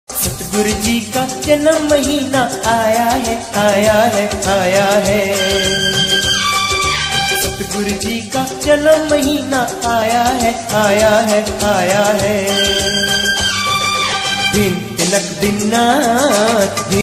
परम पिता शाह सतनाम सिंह जी महाराज जी के जन्म महा करोड़ों-करोडों बार बधाई हो जी
आप सभी को परम पिता शाह सतनाम सिंह जी महाराज जी के जन्म महा की पुज्य पिता जी ,शाही परिवार व सारी साध-संगत जी को करोड़ों-करोडों बार बधाई हो जी।🙏